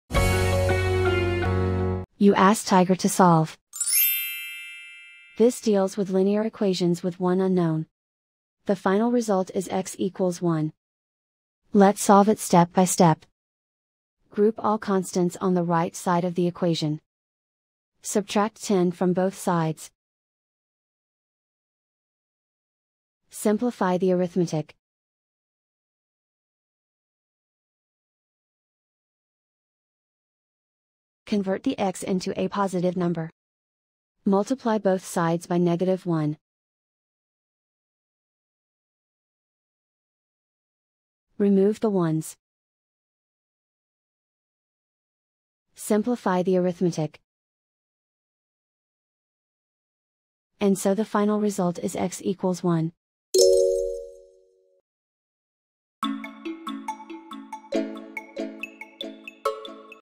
Solve -x+10=9: Linear Equation Video Solution | Tiger Algebra
Struggling with -x+10=9? Watch our step-by-step video on Tiger Algebra to master this linear equation effortlessly.